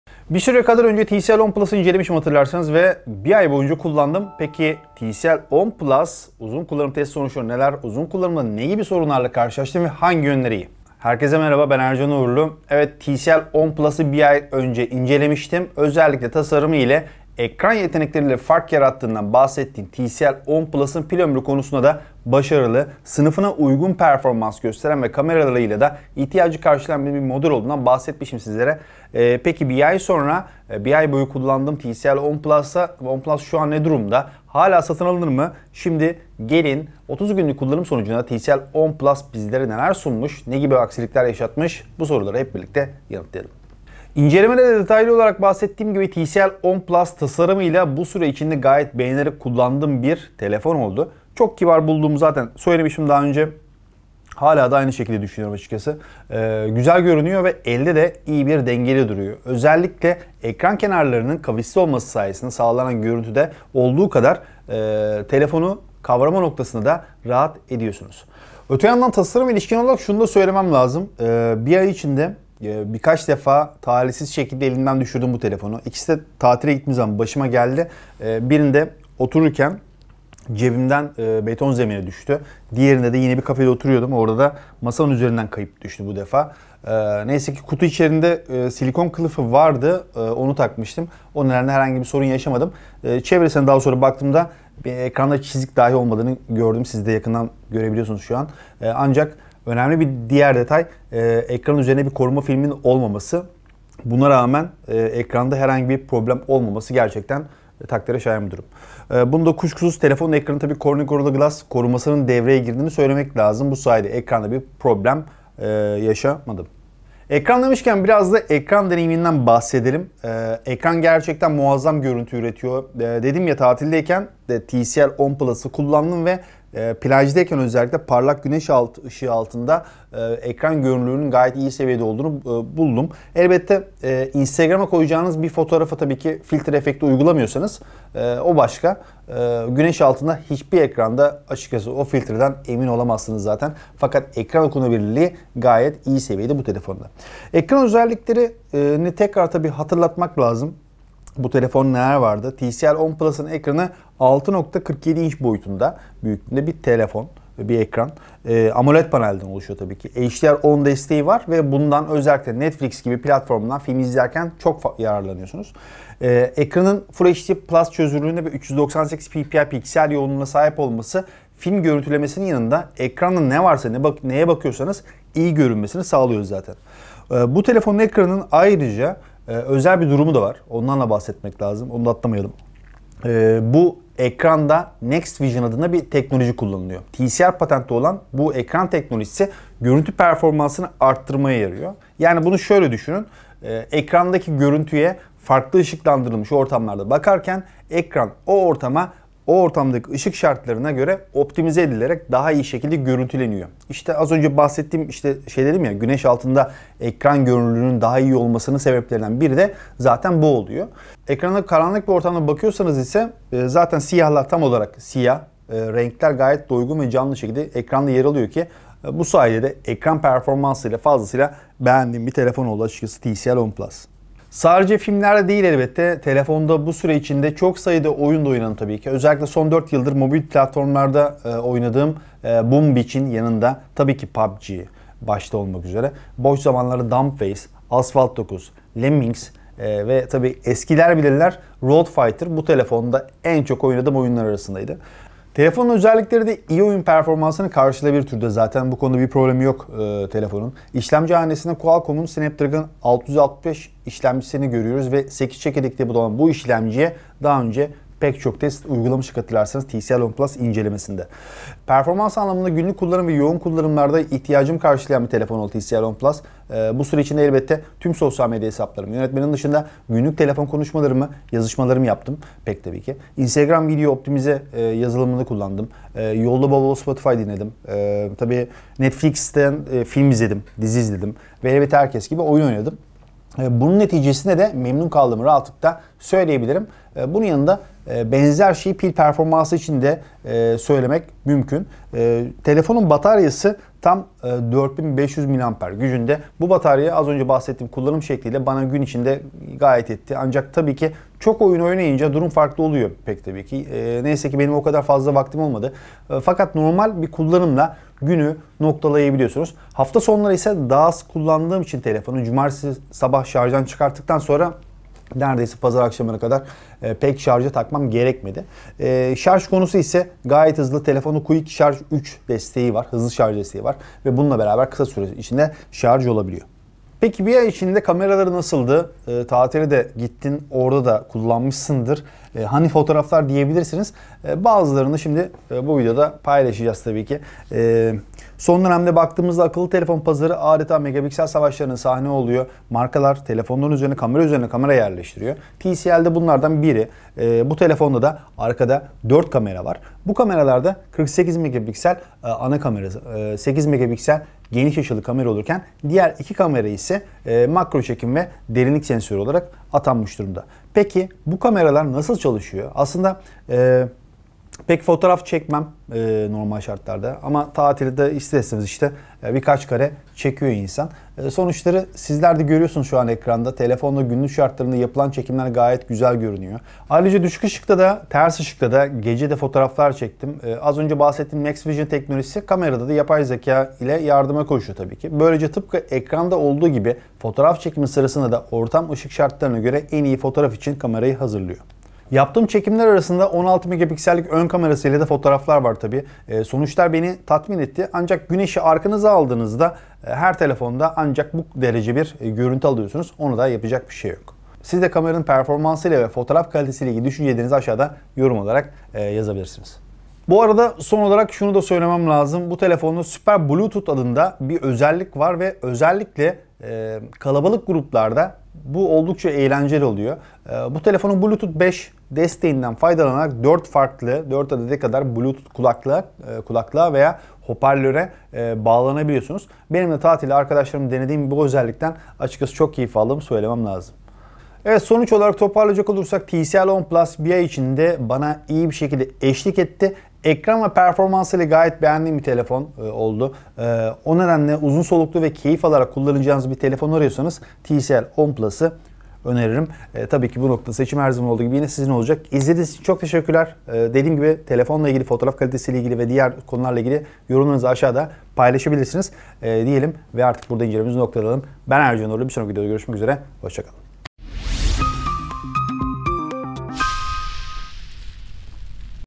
TCL 10 PLUS'I 1 AY BOYUNCA KULLANDIM | SATIN ALINIR MI?
TCL 10 Plus'ı inceledikten sonra 1 ay boyunca kullandık. Peki, TCL 10 Plus uzun kullanım testi sonuçları neler, uzun kullanımda ne gibi sorunlarla karşılaştık ve hangi yönleri iyi? İşte TCL 10 Plus uzun kullanım testi!

Evet, TCL 10 Plus'ı 1 ay kadar önce incelemiştim. Özellikle tasarımı ile ekran yetenekleriyle fark yarattığından bahsettiğim TCL 10 Plus'ın pil ömrü konusunda da başarılı, sınıfına uygun performans gösteren ve kameralarıyla da ihtiyacı karşılayan bir model olduğundan bahsetmiştim. Peki, 1 ay boyu kullandığım TCL 10 Plus ne durumda? Hala alınır mı? Şimdi gelin, 30 günlük kullanım sonucunda TCL 10 Plus bizlere neler sunmuş, ne gibi aksilikler yaşatmış; bu soruları hep birlikte yanıtlayalım.

______Video Bölümleri______

00:00 Giriş
00:44 Tasarım: TCL 10 Plus Yere Düşerse Ne Olur?
01:57 Ekran: TCL 10 Plus Netflix Deneyimi Nasıl?
03:44 Performans: TCL 10 Plus Alınır mı?
05:27 Kamera: TCL 10 Plus ile Nasıl Fotoğraflar Çektik?
06:50  Bu da Bonusu
07:15 Sonuç

TCL, bir süre önce kendi adıyla akıllı telefonlarını ülkemizde satışa sunmaya başladı ve birbiri ardına da yeni modeli duyurarak kullanıcıların karşısına çıktı. Kısa bir süre önce de akıllı telefon ailesine yeni modeller katan şirket, TCL 10 serisi ile beraber ürün yelpazesini genişletiyor. Son olarak TCL 10 Plus ve TCL 10 SE modellerini tanıtan TCL Mobile, böylece TCL 10 Pro ve TCL 10 5G modellerinden sonra aileye hareket kattı. Bugün bu modellerden TCL 10 Plus modelini inceleyeceğiz.

Özellikleri dahilinde orta segment akıllı telefonların bir üyesi olarak karşımıza çıkan TCL 10 Plus, tasarımıyla amiral gemisi görünümü sunuyor ve arka tarafta 4'lü kamera kurulumu bulunduruyor. Buna ilave olarak TCL Mobile'ın NXTVISION ekran teknolojisinin de sahibi olan telefon böylece oldukça güzel bir görüntü sunuyor. Hepsinden bahsedeceğiz, ancak önce tasarımıyla incelemeye başlayalım.

TCL 10 Plus Teknik Özellikler

İşletim Sistemi: Android 10
İşlemci: Qualcomm Snapdragon 665
Çekirdek:  8
İşlemci Hızı: 2.0 GHz & 1.8 GHz
Ekran Teknolojisi: AMOLED
Ekran Boyutu: 6.47 inç
Ekran Çözünürlüğü: 1080×2340 px
Bellek (RAM): 6 GB
Grafik İşlemci: Adreno 610
Dahili Depolama: 64 GB
Arttırılabilir Hafıza: Var
Ön Kamera: 16 MP
Arka Kamera: 48 MP & 8 MP & 2 MP & 2 MP
Batarya: 4500 mAh
Çift SIM Desteği: Var
Wi-Fi: 802.11ac
Bluetooth: 5.0
GPS: Var
LTE: Var
NFC: Var
FM Radyo: Yok
Parmak İzi Sensörü: Var
Yüz Tanıma: Var
Boyutlar: 72 x 158 x 8.9 mm
Ağırlık: 172 gr.

Ayrıca hiç beklenmedik zamanlarda ilginç vLog'lar da karşınıza çıkabilir!